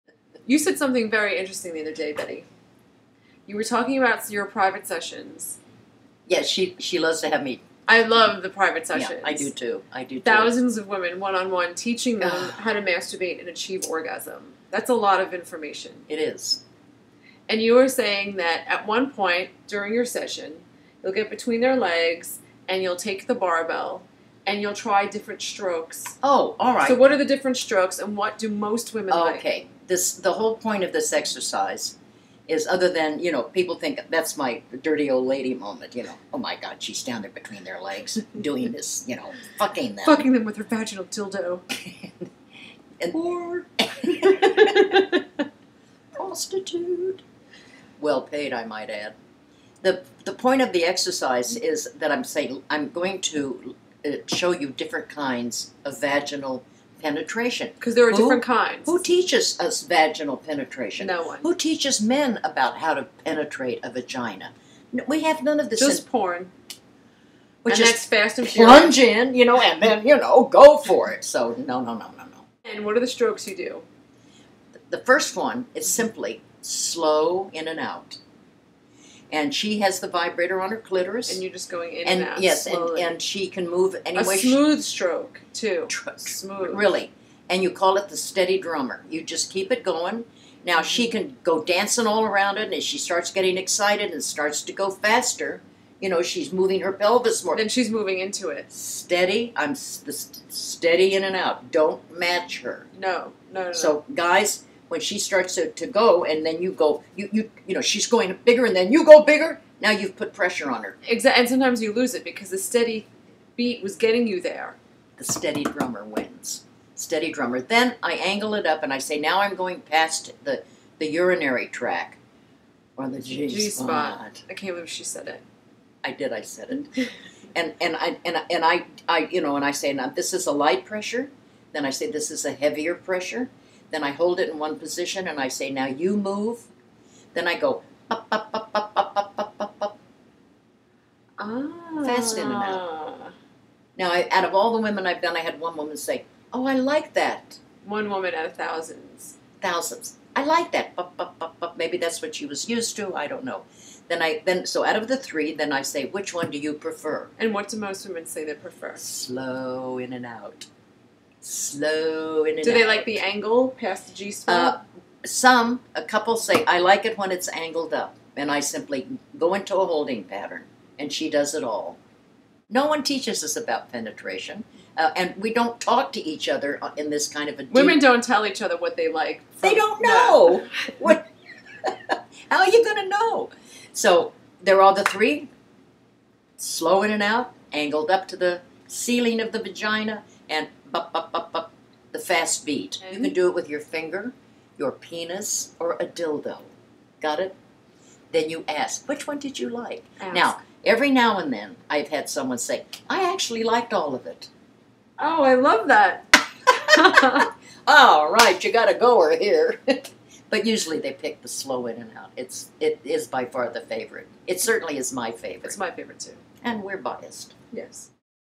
Betty & Carlin's Preference for Vaginal Penetration
I finally got Betty to talk about her private sessions.  Turns out most women like the steady drummer.